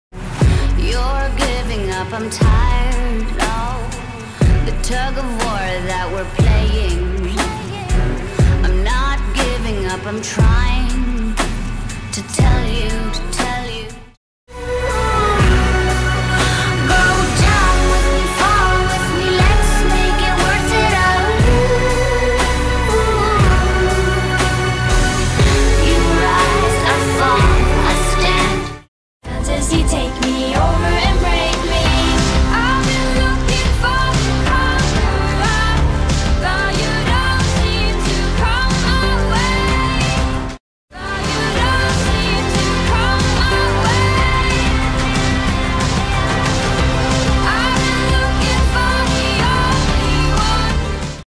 Conquer your black sea [ MEP PARTS ]
Haven't uploaded picture editing in a bit so here we go

These parts are kinda meh in general, but I feel like I managed to try a little bit more atmospheric editing + I got to edit with some characters I rarely edit with so I still got something out of it! Now I just need to try to improve my pancrop for this kind of editing cause it's a mess honestly. xD

ぬるはち (Pixiv ID: 32796452)